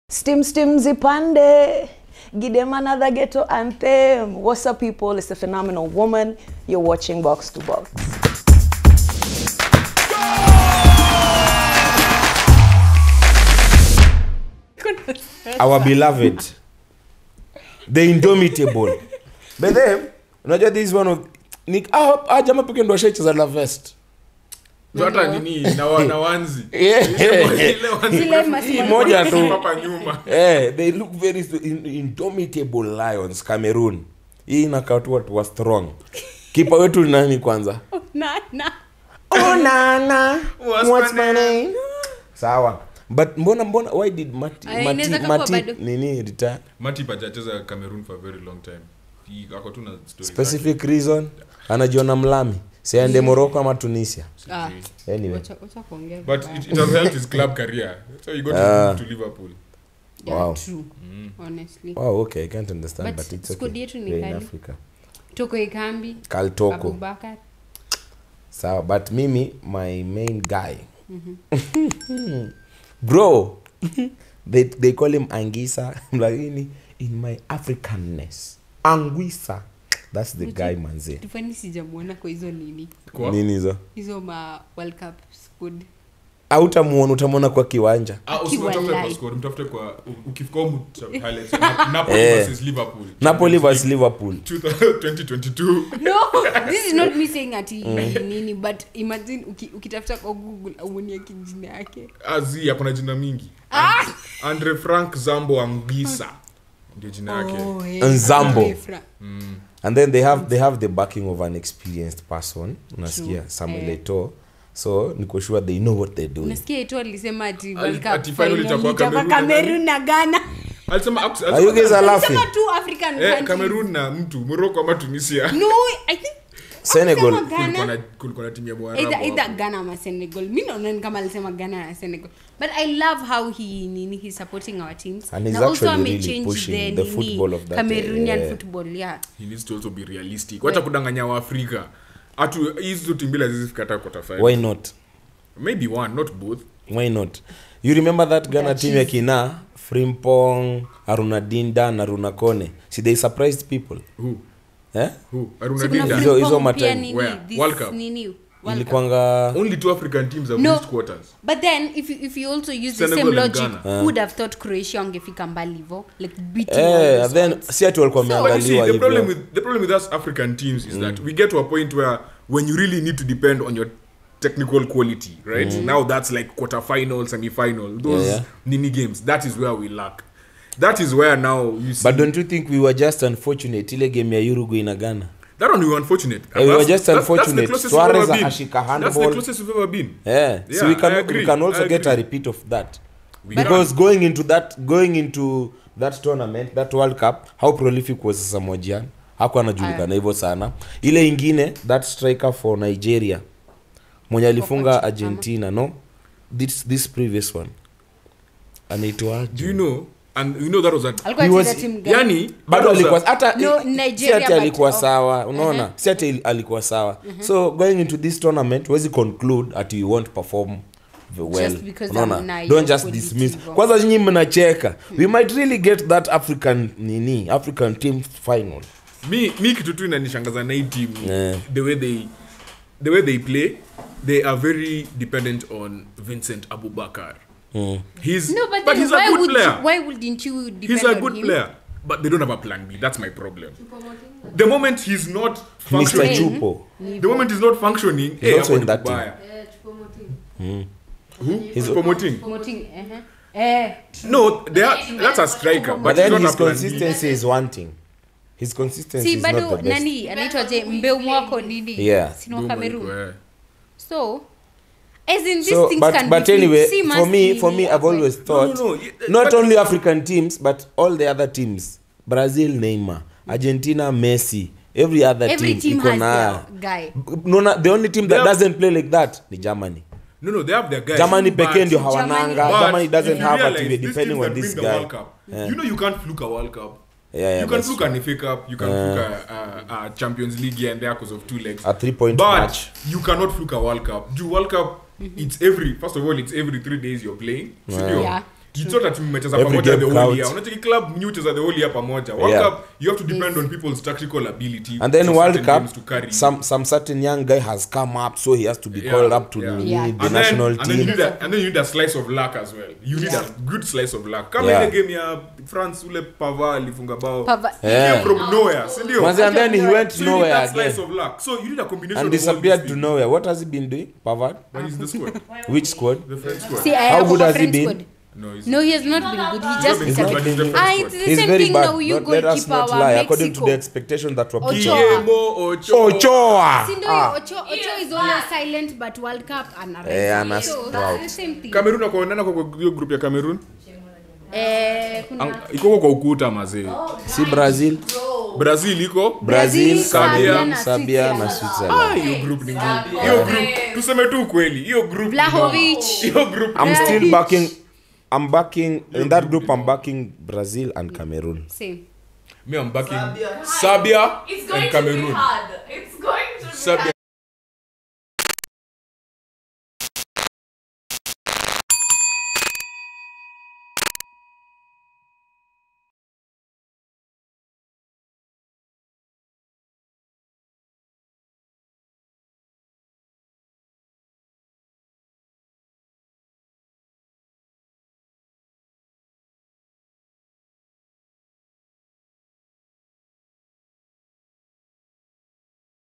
CAMEROON | AFRICA AT THE WORLD CUP
How far can Cameroon go at the World Cup?

5-part series where we preview all 5 African teams going to Qatar.

Produced by: Tino Mudiwa
Shot and edited by: Joshy Marvin